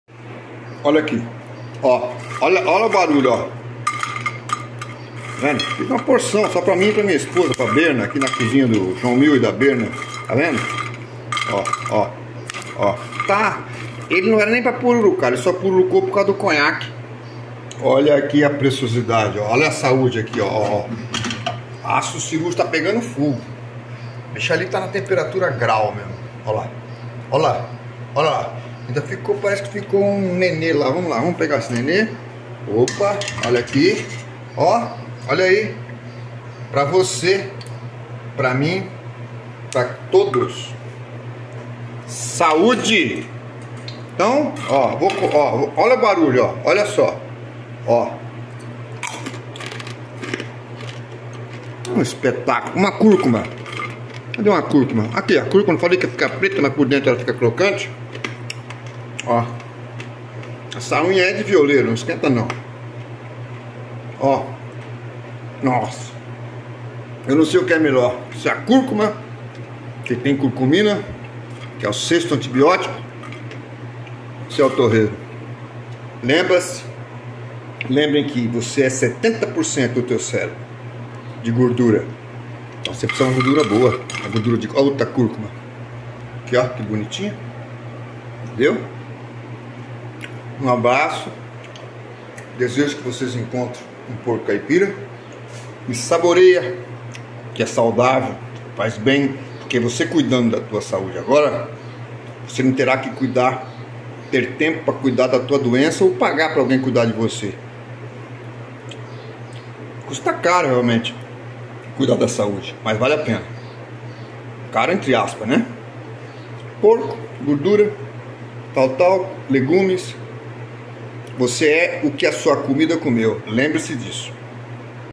FECHOU COM CHAVE DE OURO
Viu como é simples e faz bem prá saúde. Sal de boi,um tiquinho de óleo de côco na panela,uns pedaços de açafrão ou cúrcuma, 1 xícara de conhaque, álcool ou cachaça pra dar um grau.E é claro um pouco de PANCETA em pedaços. Ta sentindo o cheirinho? Dá 1LIKEMILESEINSCREVAI...me fala dessa maravilha que é saúde. @canaltorrei